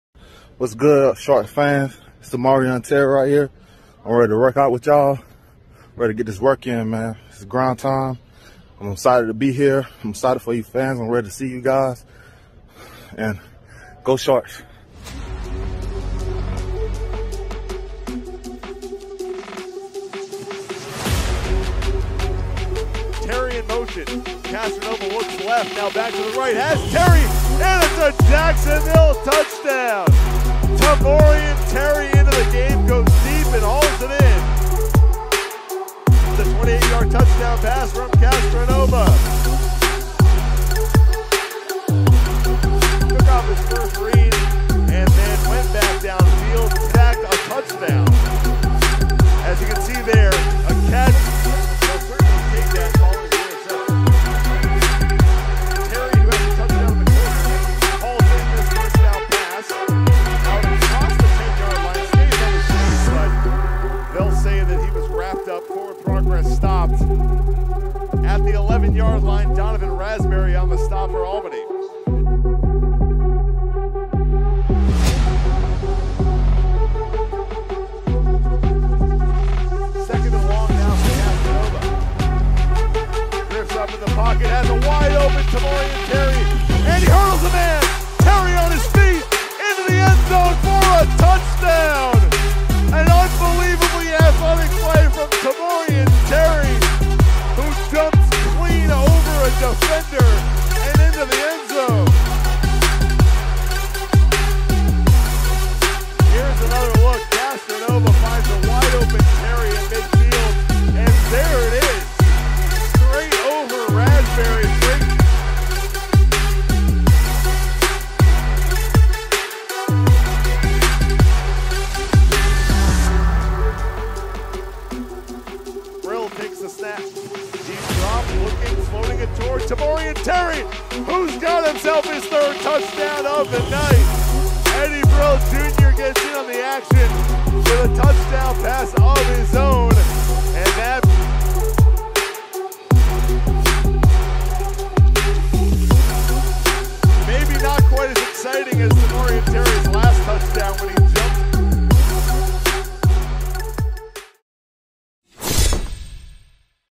Tamorrion Terry (WR) Makes His NAL Debut 5/20/23
Terry was a 3 star recruit out of Turner County High School in Ashburn, GA. Class A Public First Team All-State in 2015 and 2016, Terry was ranked the No. 32 receiver in the country by 247Sports. He signed with FSU over offers from Florida, Georgia, Ole Miss, and Auburn.

During his time as a Seminole, Terry had 118 receptions for 2,221 yards and 18 touchdowns. His 2,221 receiving yards placed him at 10th all-time in FSU history while his 18 touchdowns tied him for 13th. Terry averaged a touchdown every 6.5 receptions and his average of 49 yards per touchdown reception was second-highest among NCAA receivers with at least 15 touchdowns over the past decade.